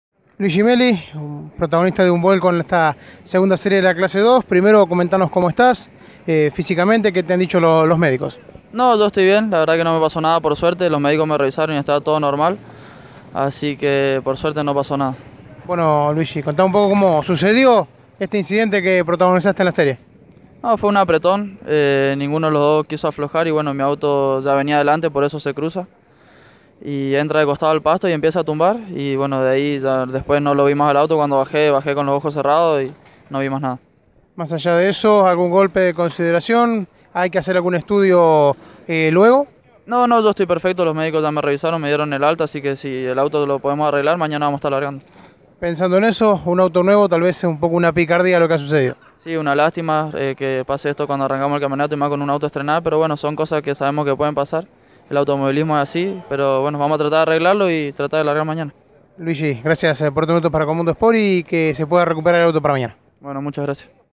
Luigi Melli - Vuelco Serie 2 Clase 2 TP Olavarría 2019
Tras protagonizar un vuelco en la segunda serie de la Clase 2 del Turismo Pista en el autódromo de Olavarría, Luigi Melli pasó por los micrófonos de Mundo Sport y confirmó que se encuentra en buen estado de salud y que intentarán recuperar el Corsa para largar la final de mañana.